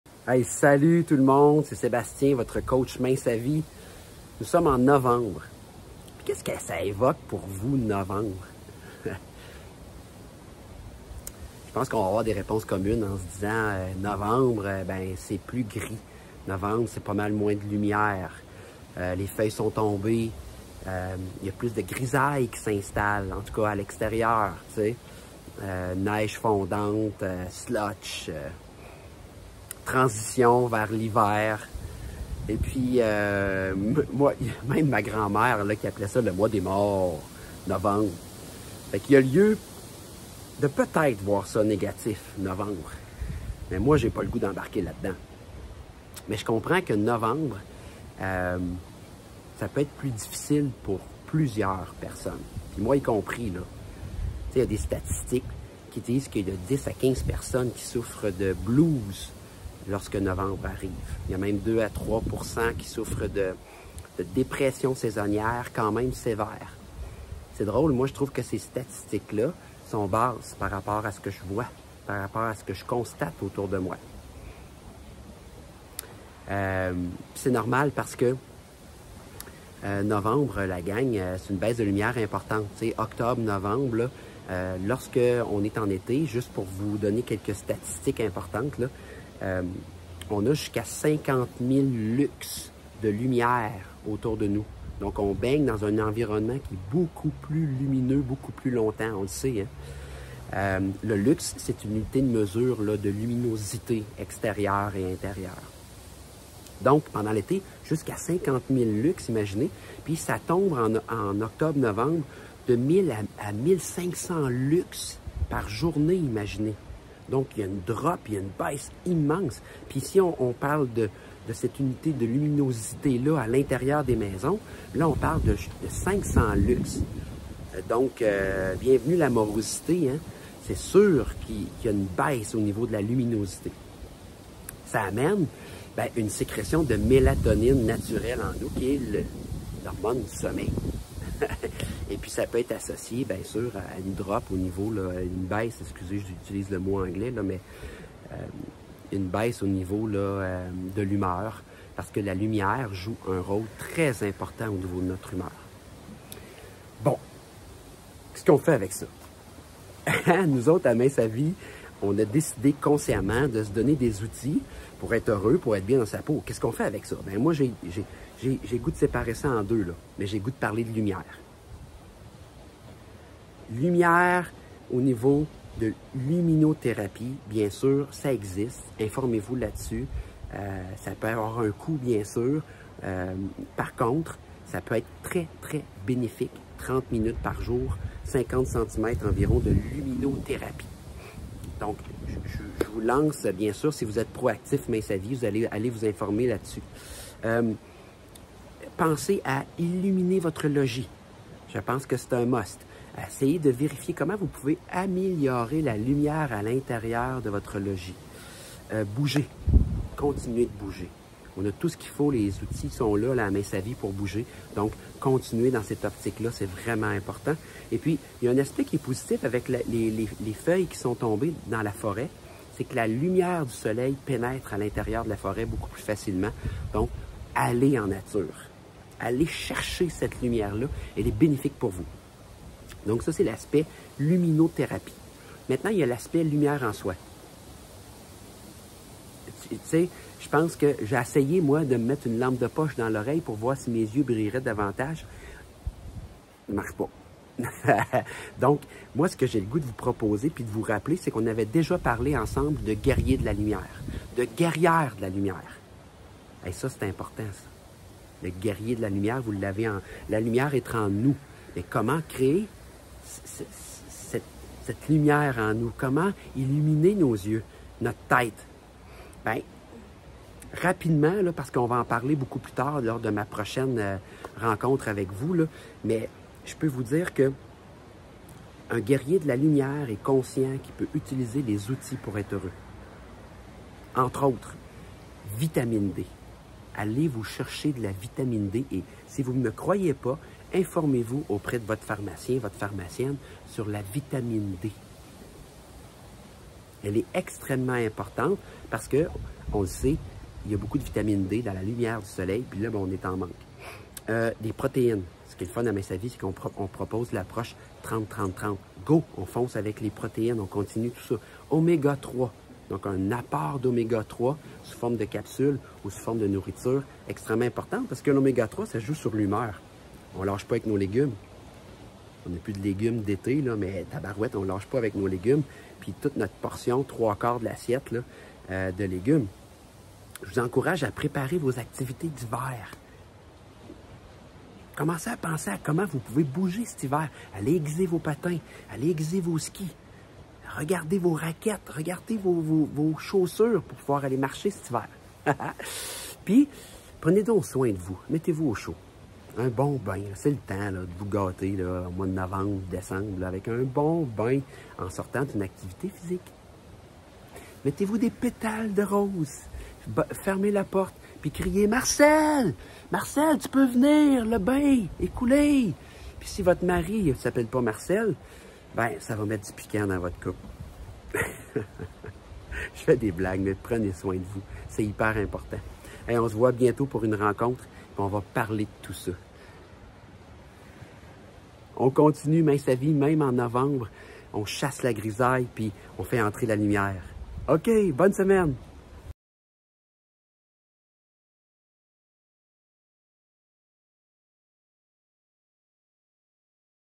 Minçavi en novembre - Réunion spéciale Mercredi 23 novembre 19h
[Réunion avec Sébastien, votre coach expert en motivation]
Minçavi en novembre ; le changement d'heure, la baisse de lumière, la grisaille fait-il en sorte que notre motivation est à son plus bas? Venez voir Sébastien Laurence en conférence et découvrir ses trucs qui vous aideront.

👉 Sébastien sera en ligne mercredi 23 novembre à 19h, soyez des nôtres!